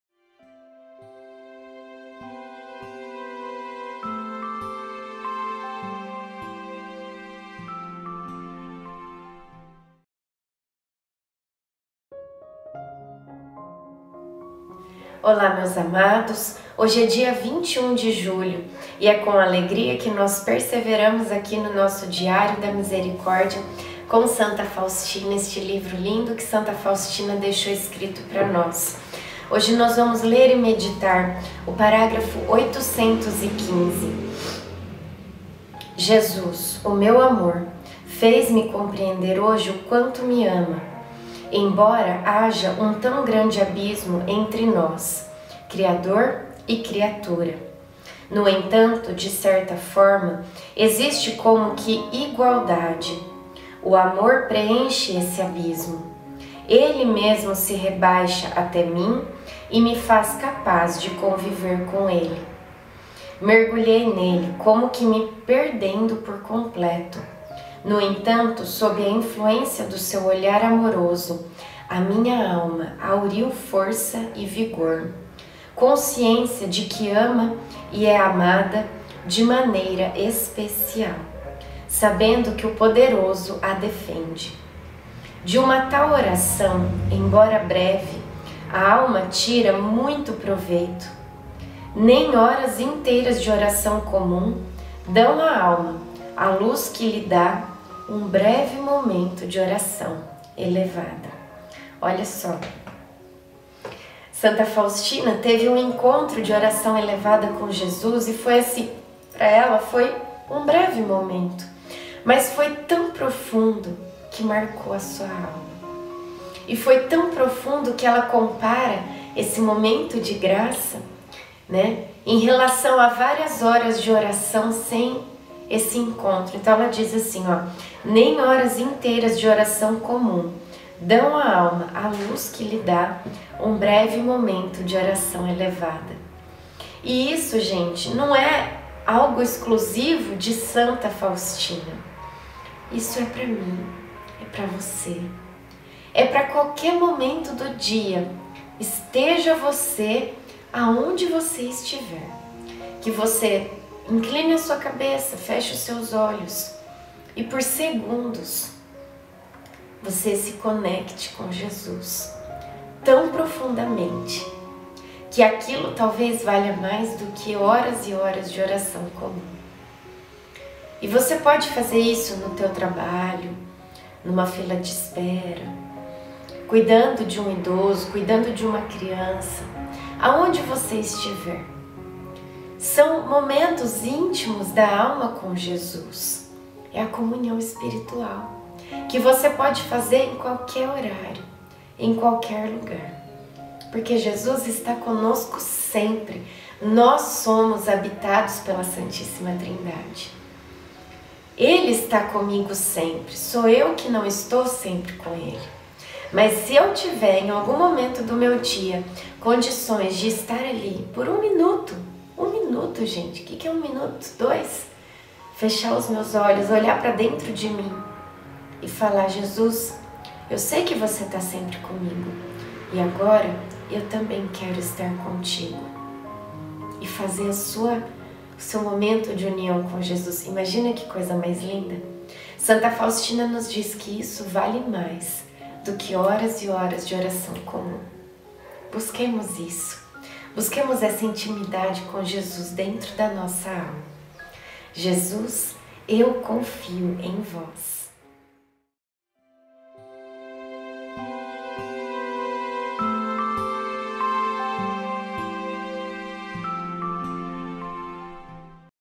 DIÁRIO DA MISERICÓRDIA COM SANTA FAUSTINA 21/07/22
815Jesus, o meu Amor, fez-me compreender hoje o quanto me ama,
embora haja um tão grande abismo entre nós: Criador e criatura.

No entanto, de certa forma, existe como que igualdade: o amor
preenche esse abismo. Ele mesmo se rebaixa até mim e me faz
capaz de conviver com Ele. Mergulhei Nele, como que me
perdendo por completo — no entanto, sob a influência do Seu olhar
amoroso a minha alma hauriu força e vigor, consciência de que
ama e é amada de maneira especial, sabendo que o Poderoso a
defende. De uma tal oração, embora breve, a alma tira muito
proveito. Nem horas inteiras de oração comum dão à alma a luz
que lhe dá um breve momento de oração elevada.

Deus abençoe!